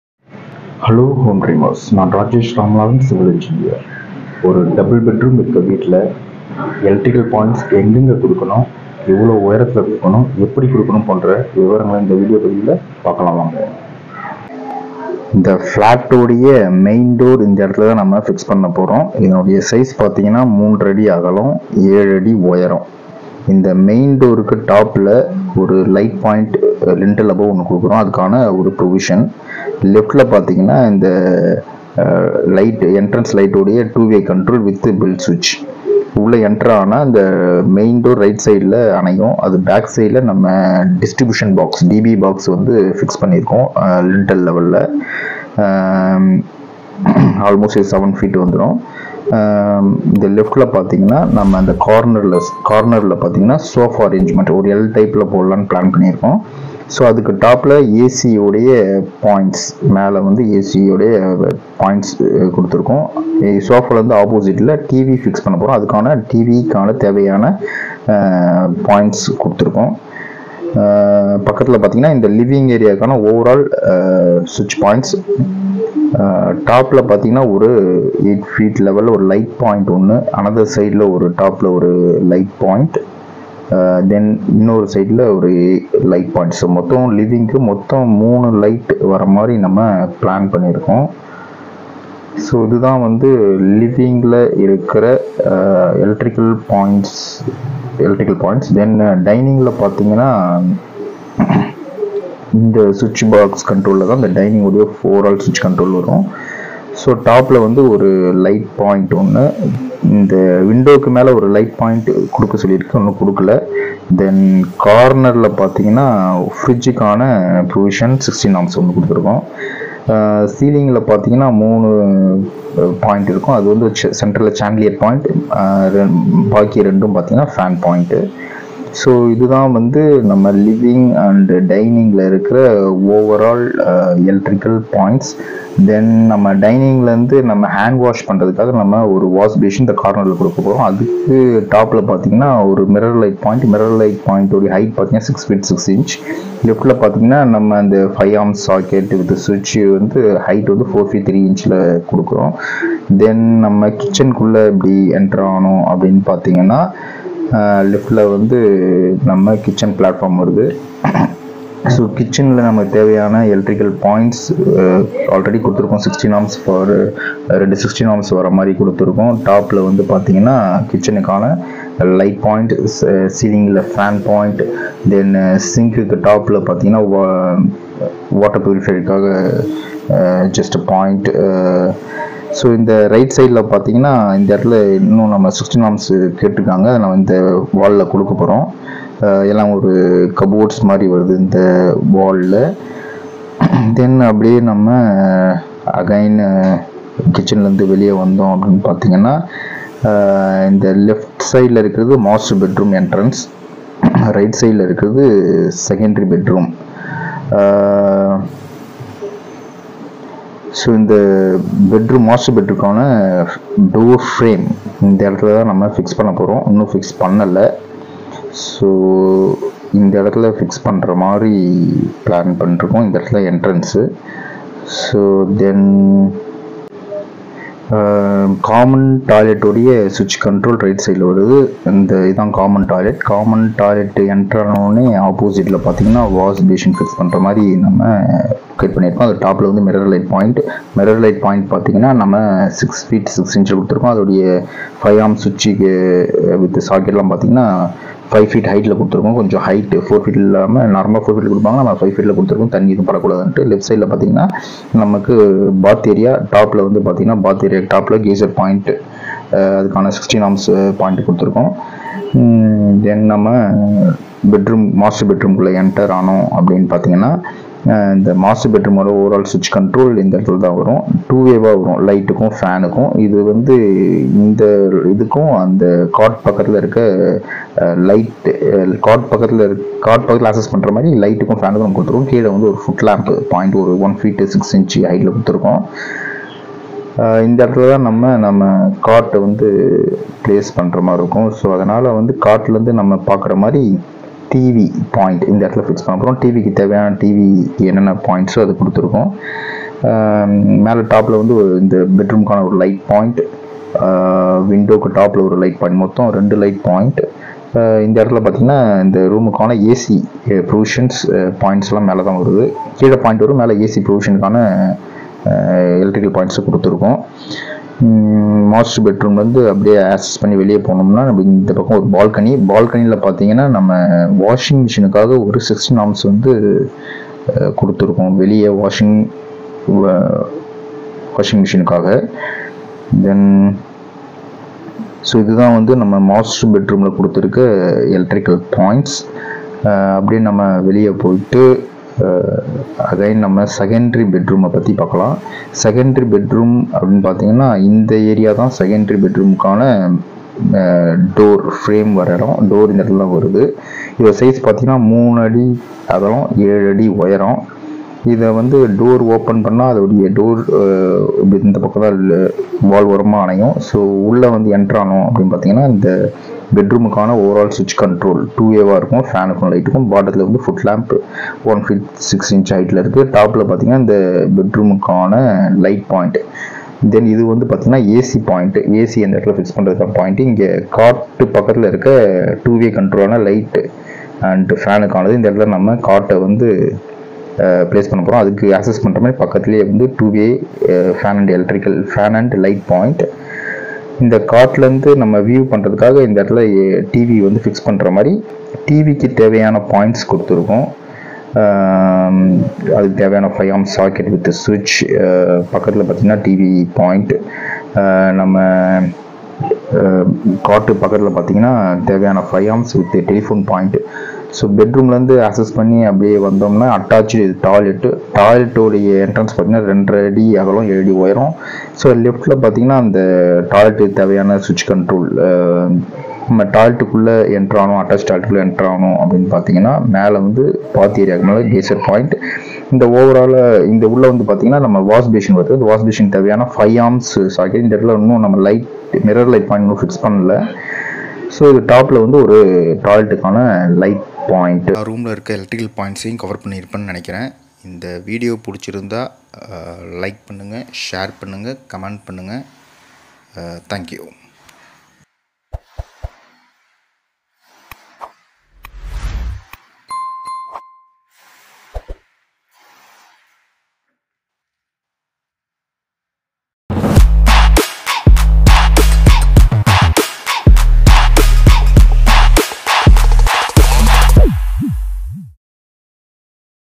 வீட்டில் எலக்ட்ரிக்கல் பாயின்ஸ் எங்கு கொடுக்கலாம் | Ambattur Site Series #20 | Er Rajesh Ramanathan
Hello Home Dreamers (HD's),

இரண்டு பெட் ரூம் உள்ள வீட்டில் எலக்ட்ரிக்கல் பாயின்ஸ் குறைந்தபட்சம் எங்கு கொடுக்கலாம்!!!